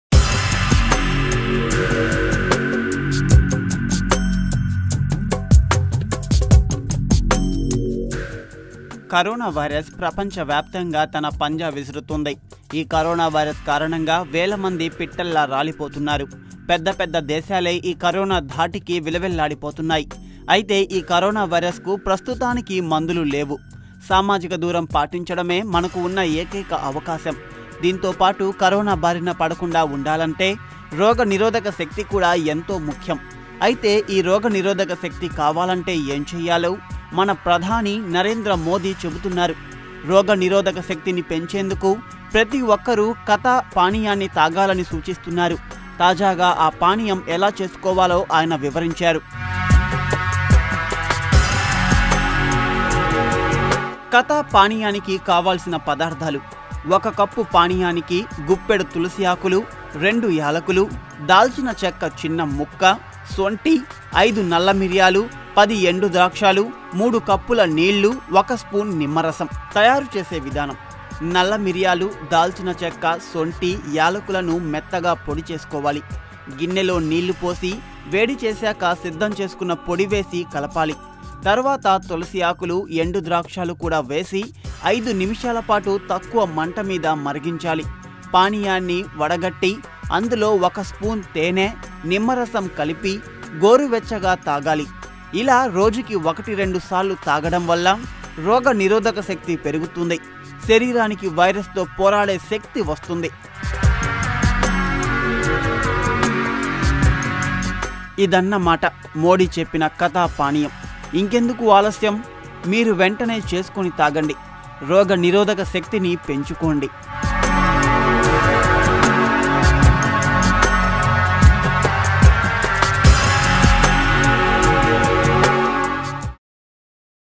ప్రధాని మోడీ చెప్పిన కడ టీ ..ఇది తాగితే రోగ నిరోధక వ్యవస్థ అద్భుతం...
ప్రధాని మోడీ చెప్పిన కడ టీ ..ఇది తాగితే రోగ నిరోధక వ్యవస్థ అద్భుతంగా ఉంటుందని , వైరస్ దాడిని ఎదుర్కొనే శక్తి వస్తుందని చెప్పాడు. కడ టి ఇలా చెయ్యాలి..